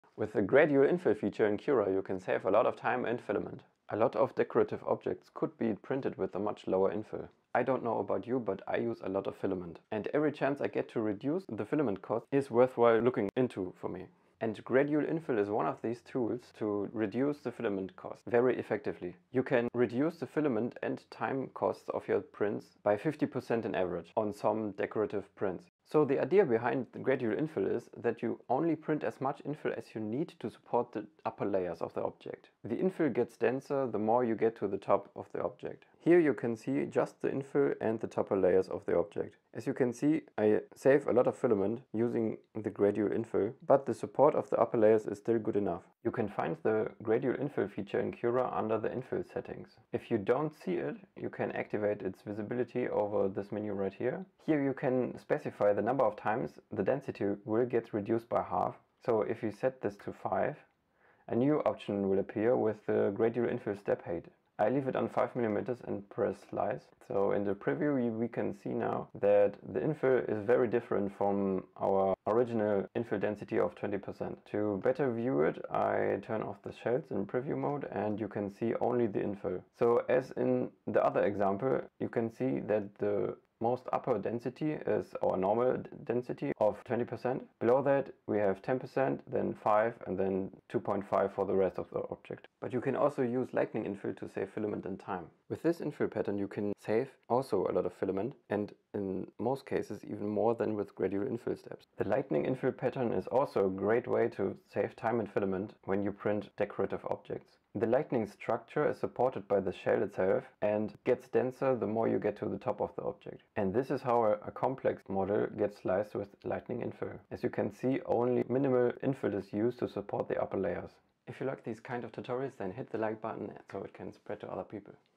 Cura Gradual Infill Guide | How to Save Filament and Time!
Gradual Infill in Cura allows an object to be printed more densely at the top than at the base of the object. This is especially worthwhile for decorative purposes to save filament and printing time.

00:00 Intro
00:14 Saving print time and filament
00:26 What is Gradual Infill?
00:46 Cura settings
01:32 Alternative
02:00 Outro

Find tips, printer profiles, tutorials and hands-on reviews of the best 3D printers